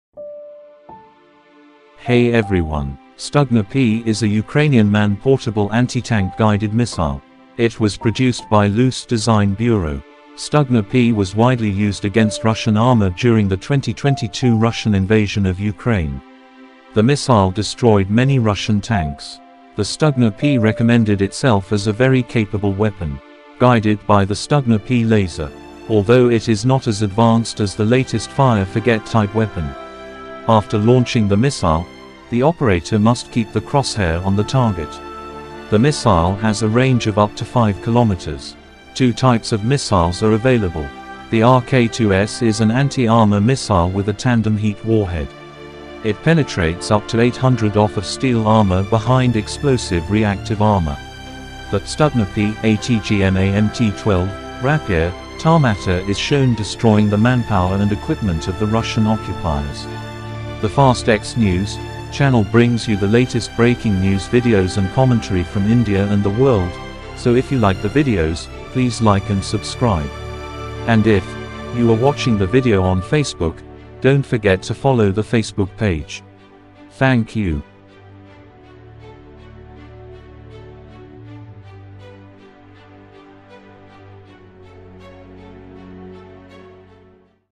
"Stutna- P" ATGM destroyed Russian troops and equipment with the help of the MT-12 "Rapir" Tarmata.
The "Stutna- P" ATGM destroyed Russian troops and equipment with the help of the MT-12 "Rapir" Tarmata.

The "Stutna- P" ATGM of the Odessa 28th Mechanized Brigade of the Ukrainian Armed Forces observed the destruction of manpower and equipment of the occupiers with the help of MT-12 "Rapir" Tarmata.

"The Fast X News" News YouTube channel brings you the latest breaking news videos and explainers from the IND and around the world.

WARNING: This video contains scenes some viewers may find disturbing.